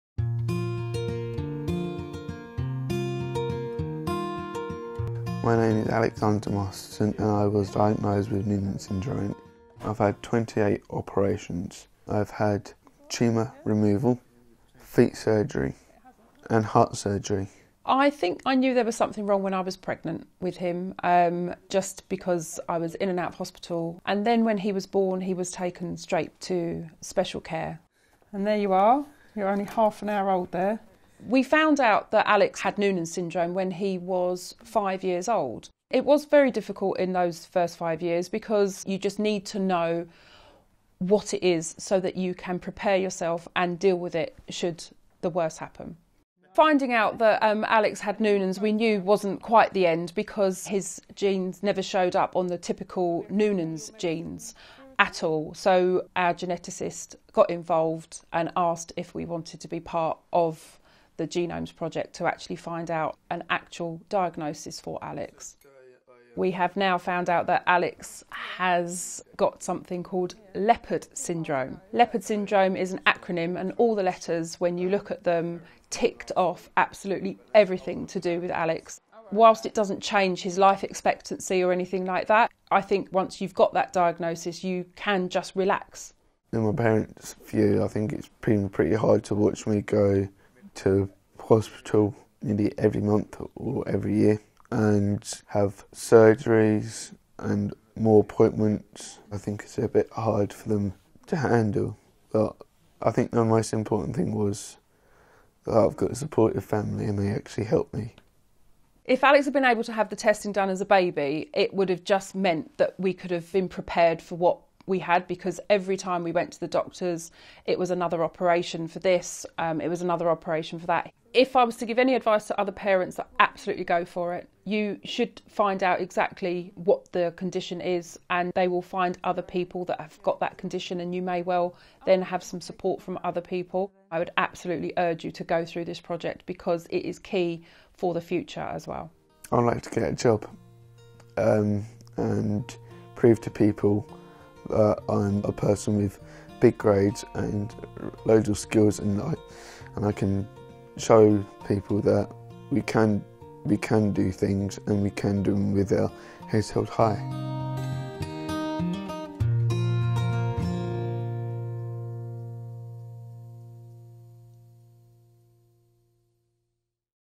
Participant Stories - Alex (subtitled)
In this film, Alex and his mum Kirsty share their story and what getting a diagnosis from the 100,000 Genomes Project means to them.